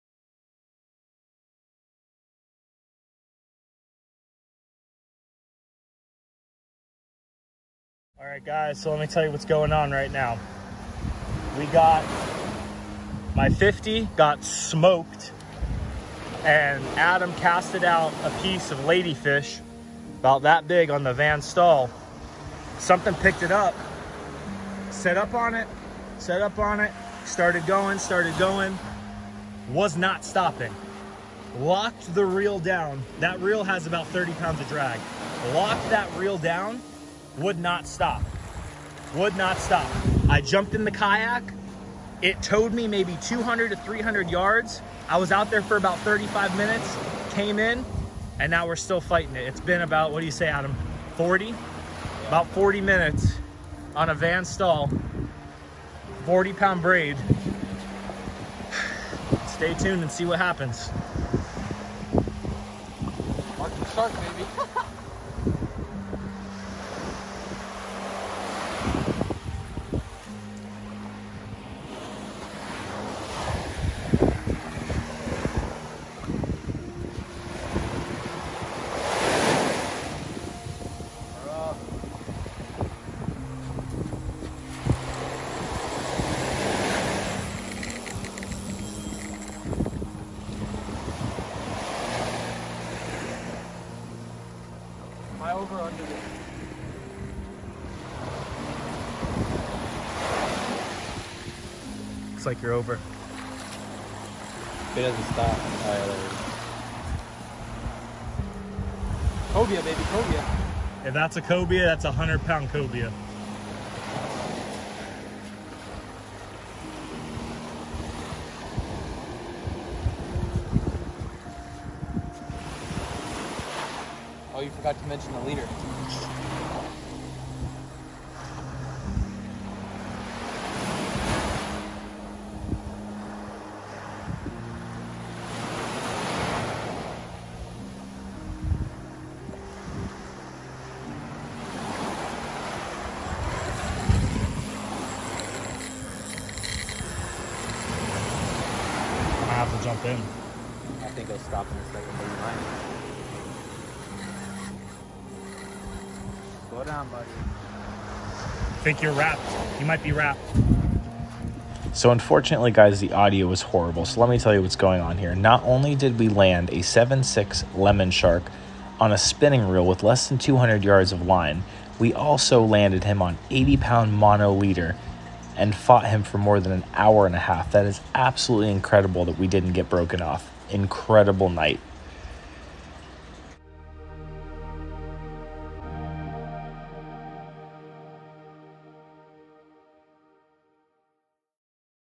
Huge Lemon SHARK on light tackle off BEACH! (got spooled)
Aggressive lemon shark nearly schools are spinning reel as it pulls us in a kayak